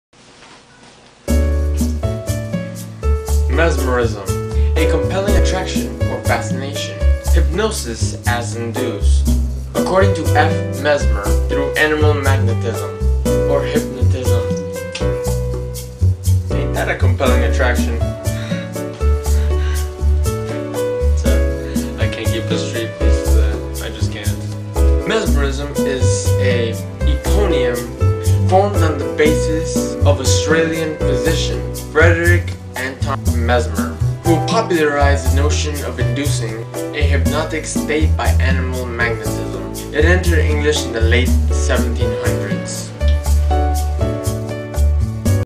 Mesmerism
Mesmerism [mes-muh-riz-uh- m, mes-]
noun
1. a compelling attraction; fascination
2. hypnosis as induced, according to F.A. Mesmer, through animal magnetism.
3. hypnotism
Mesmerism is an eponym formed on the basis of Australian physician Friedrich Anton Mesmer, who popularized the notion of inducing a hypnotic state by animal magnetism. it entered English in the late 1700s.
The word was provided by Dictionary.com